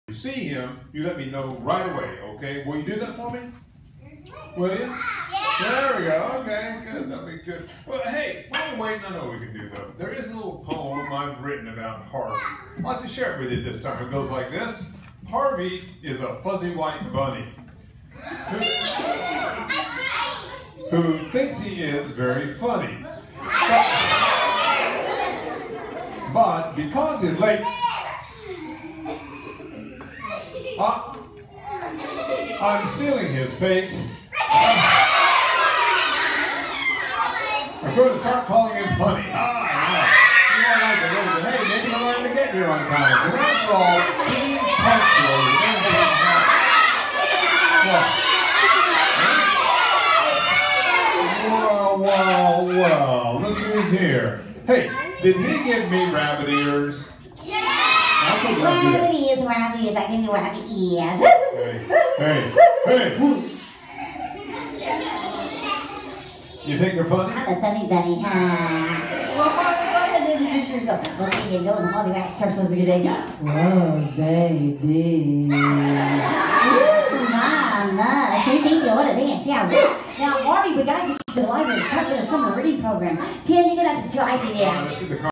Library patrons delight in return of ‘Harvey Rabbit & Friends Show’
Tim and Laura Allured, creators of “The Harvey Rabbit & Friends Show,” delight library patrons with their one-hour comedic educational act of ventriloquism, puppetry, music, magic, and audience participation.